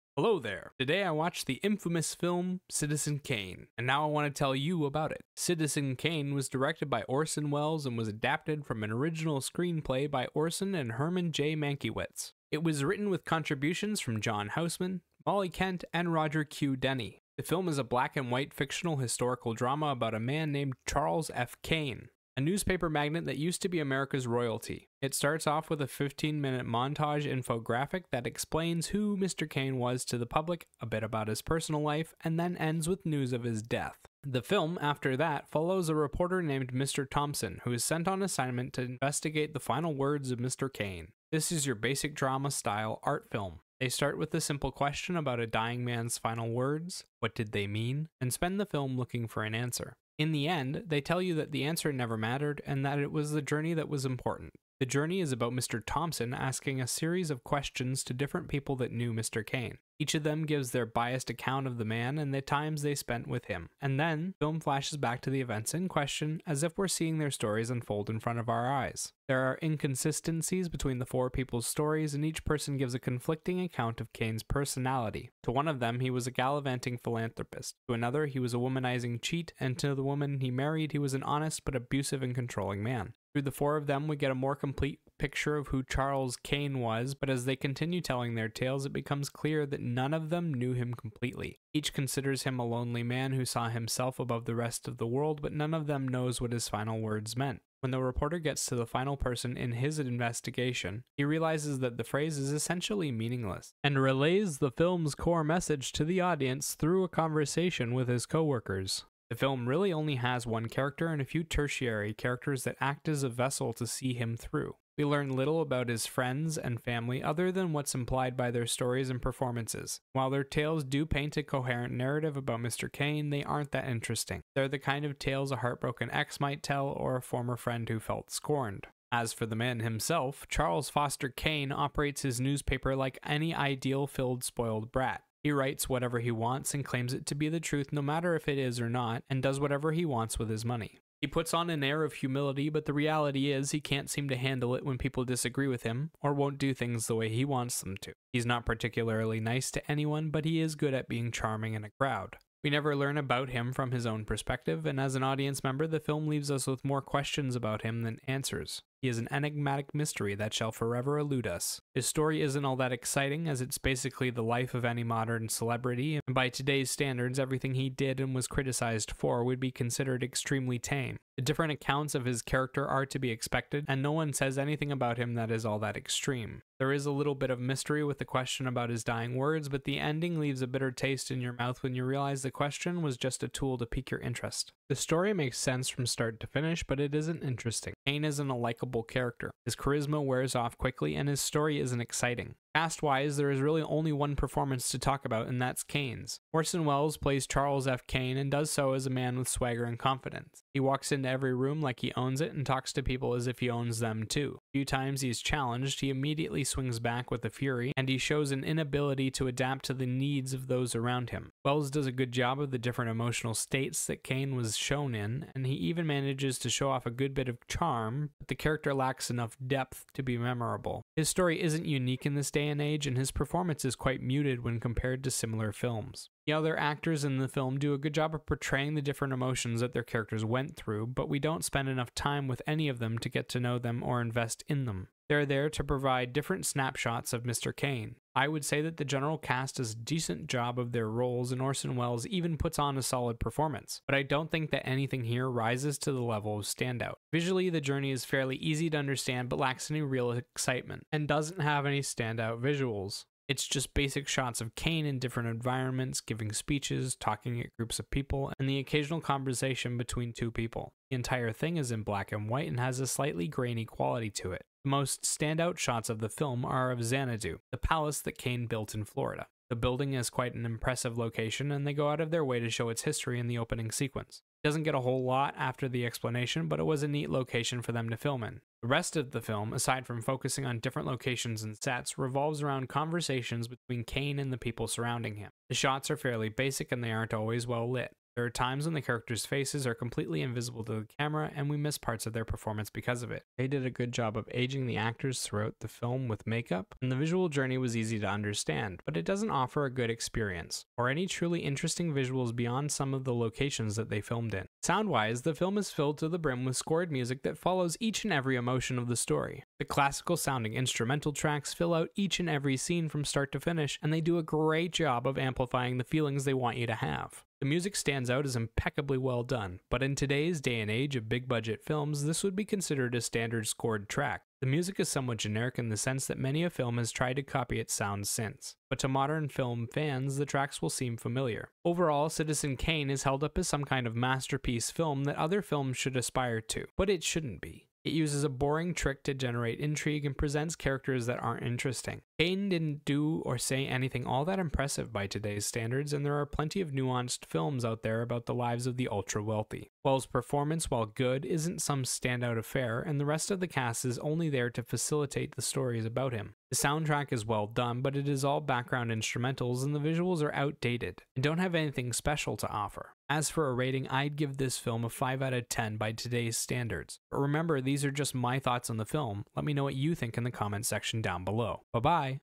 Citizen Kane (1941) viewed through a modern lens
This is my review of the film citizen Kane. It's set to shots from the film, and covers the writing, acting, visuals, and soundtrack. It also gives a rating at the end. I try to release five reviews a week and cover a plethora of new content and one old movie a week.